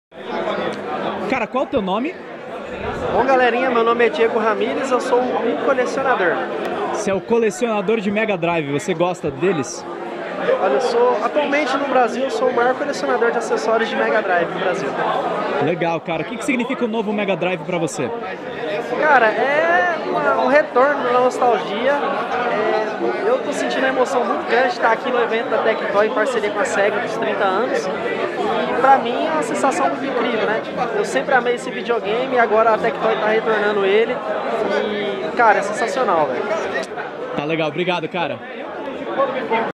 Entrevista com Ucolecionador no evento da Tectoy
O editor Pedro Zambarda entrevista Ucolecionador da SEGA, chamado Diego Ramires.